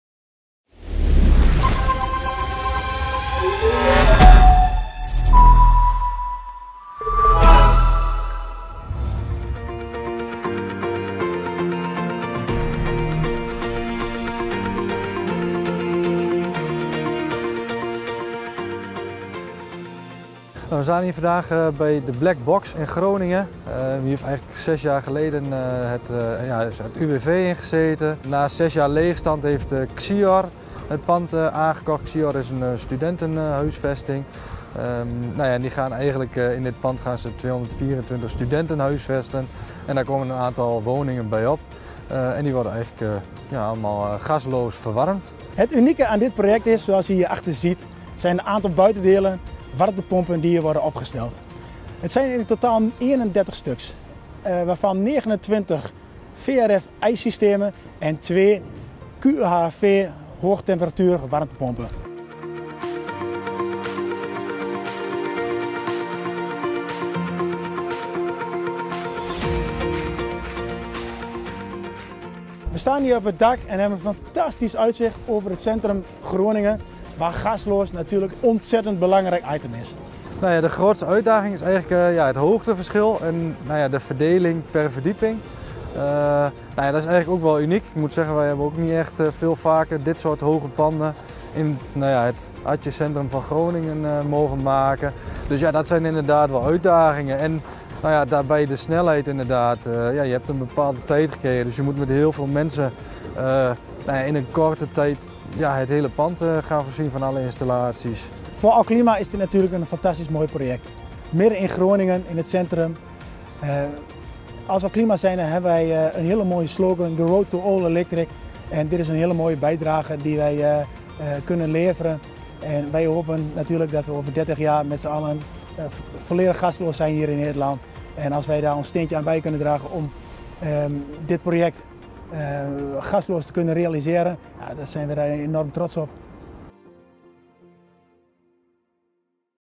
Transformatie van voormalig UWV pand naar duurzame studentenhuisvesting in Groningen
Ontdek de opmerkelijke transformatie van het voormalige UWV-pand in Groningen tot duurzame studentenhuisvesting. Na 6 jaar leegstand is het gebouw overgenomen en zal het 224 studenten huisvesten, samen met extra gasloze woningen. Het project omvat 31 buitendelen, waaronder 29 VRF Y-systemen en 2 QAHV hoge temperatuur warmtepompen. Het unieke hoogteverschil en de stedelijke omgeving brengen uitdagingen met zich mee, maar Alklima zet zich in voor deze duurzame verandering met hun slogan "The Road To All-Electric". Een stap richting een gasloze toekomst in Nederland.

Benieuwd naar wat we nog meer doen?